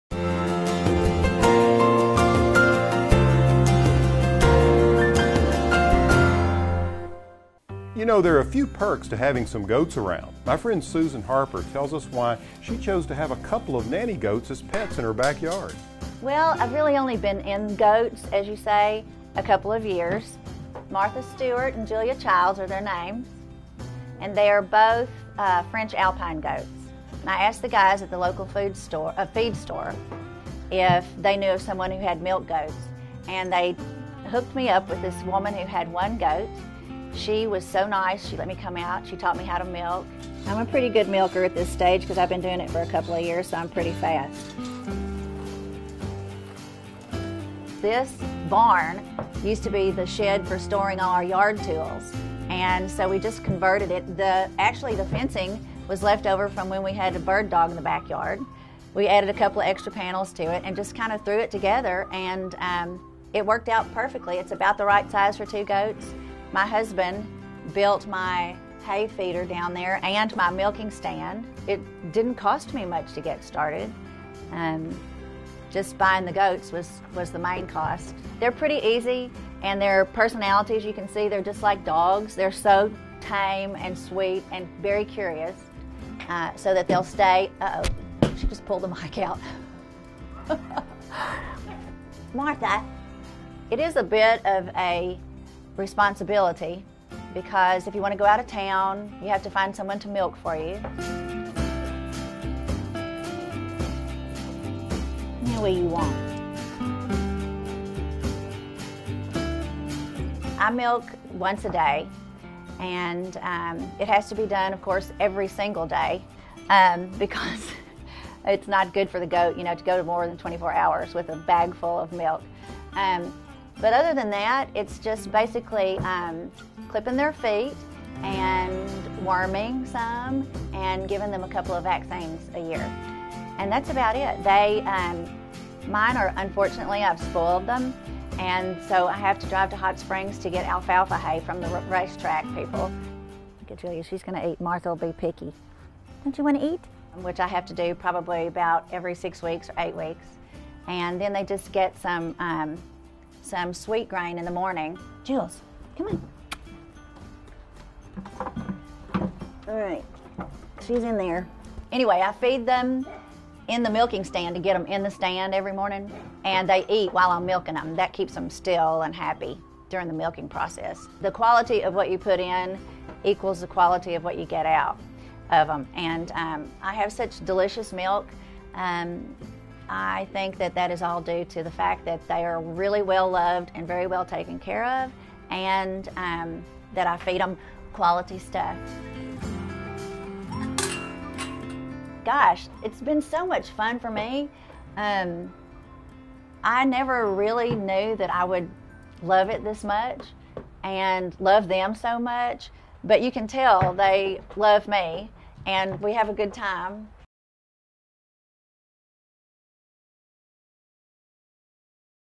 Raising Backyard Goats
Arkansan and backyard goat farmer Susan Harper shares with us the joys of raising goats.

You know, there are few perks to having some goats around. My friend, Susan Harper, tells us why she chose to have a couple of nanny goats as pets in her backyard.

Well, I've really only been in goats, as you say, a couple of years. Martha Stewart and Julia Childs are their names.  And they are both French Alpine Goats.  And I asked the guys at the local food store -- feed store if they knew of someone who had milk goats, and they hooked me up with this woman who had one goat. She was so nice, she let me come out, she taught me how to milk.
 I'm a pretty good milker at this stage 'cause I've been doing it for a couple of years, so I'm pretty fast.

This barn used to be the shed for storing all our yard tools. And so, we just converted it. Actually, the fencing was left over from when we had a bird dog in the backyard.  We added a couple of extra panels to it and just kind of threw it together, and it worked out perfectly. It's about the right size for two goats. My husband built my hay feeder down there and my milking stand. It didn't cost me much to get started.

And just buying the goats was the main cost. They're pretty easy, and their personalities, you can see they're just like dogs. They're so tame and sweet and very curious. So that they'll stay -- uh-oh. She just pulled the mike out.  Martha.
 It is a bit of a responsibility
Because if you want to go out of town, you have to find someone to milk for you. Now what do you want? I milk once a day, and it has to be done, of course, every single day because it's not good for the goat, you know, to go more than 24 hours with a bagful of milk but other than that, it's just basically clipping their feet and worming them and giving them a couple of vaccines a year. and that's about it. they -- mine are, unfortunately, I've spoiled them and so, I have to drive to hot springs to get alfalfa hay
from the race-track people.